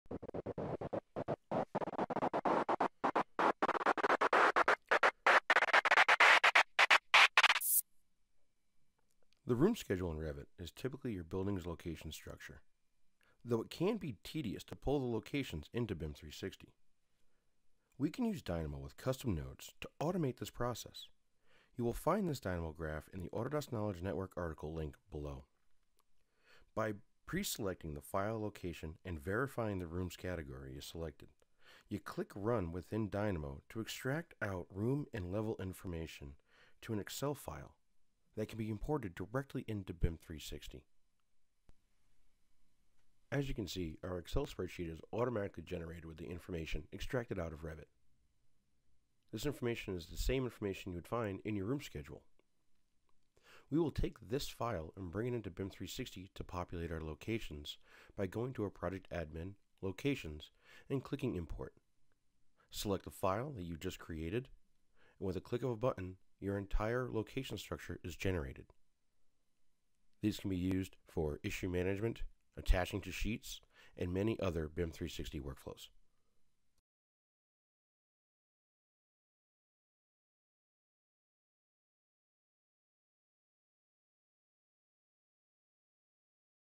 Tech HUB Tip: Bring Revit Room Information into BIM 360 with Dynamo
How to easily bring your Revit Room and Level information into BIM 360 using Dynamo.